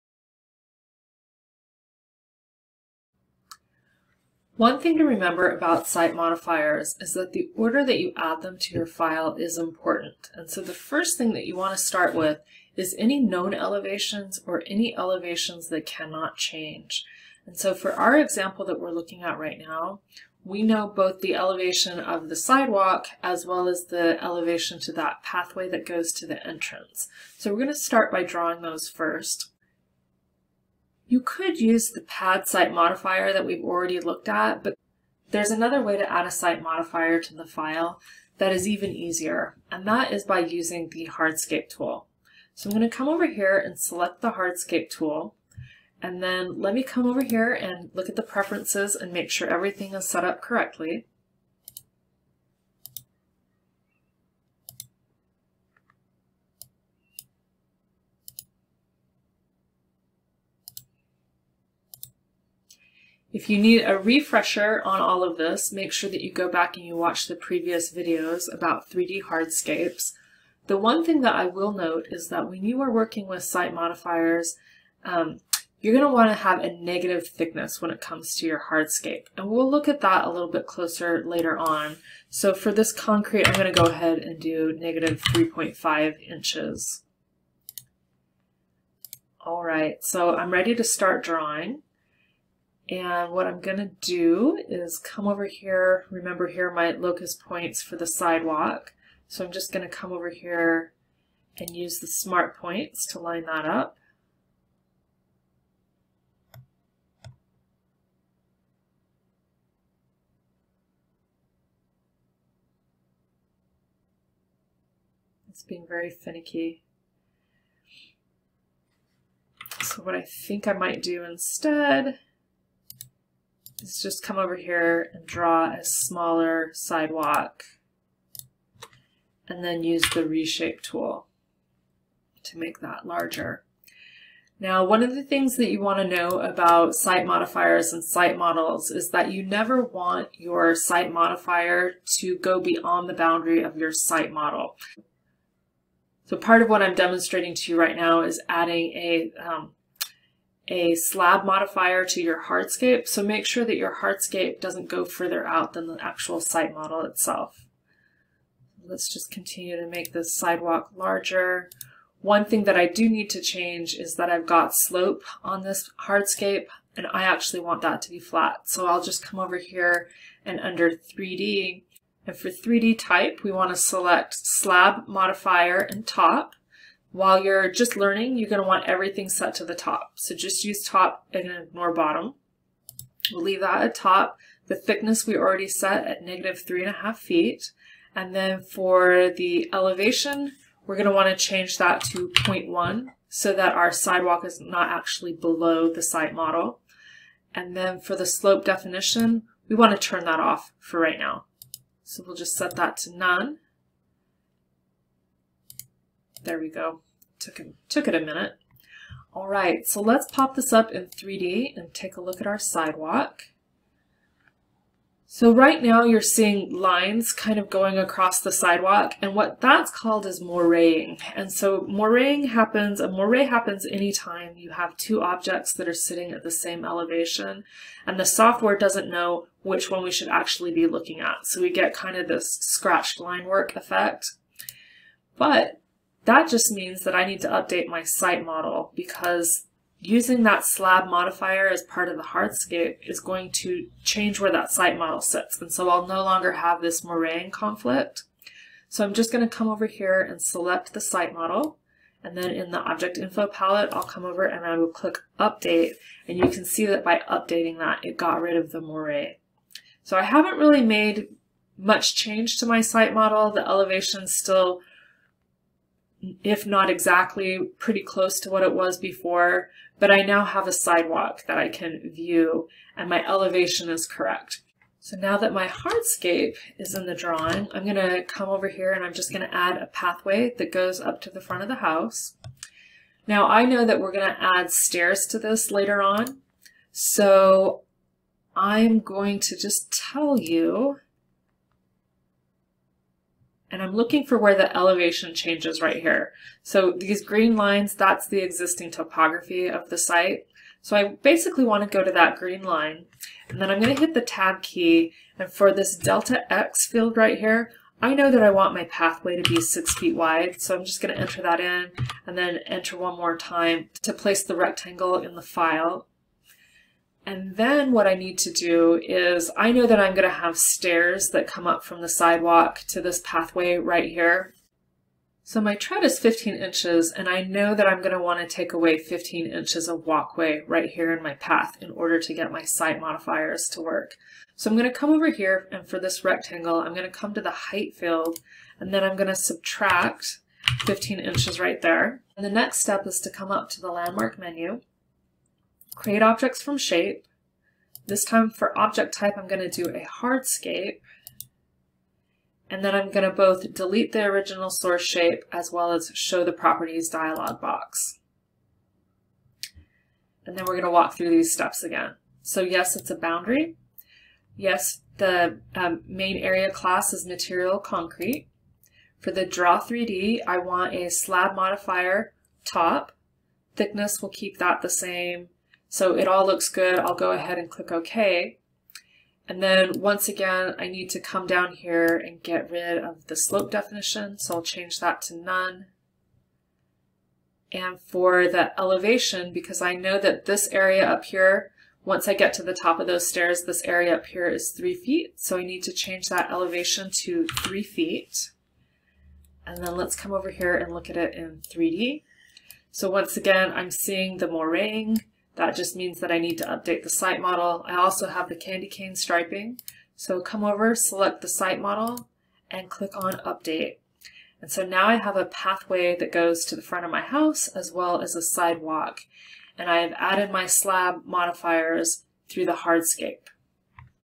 The easiest and first modifiers to add - Hardscape Slab Modifiers - Vectorworks Site Modifiers 2 of
Hardscape Slab modifiers work the same way pad modifiers work only they are directly linked to a hardscape object. This prevents any misalignment issues.

In the Object Info Palette there is a 3D Section where the type can be set to Slab with Modifier. Slab with Top Modifier is recommended unless you need to show the full thickness of the material.

This video is part of series demonstrating how to create site models using Vectorworks Landmark.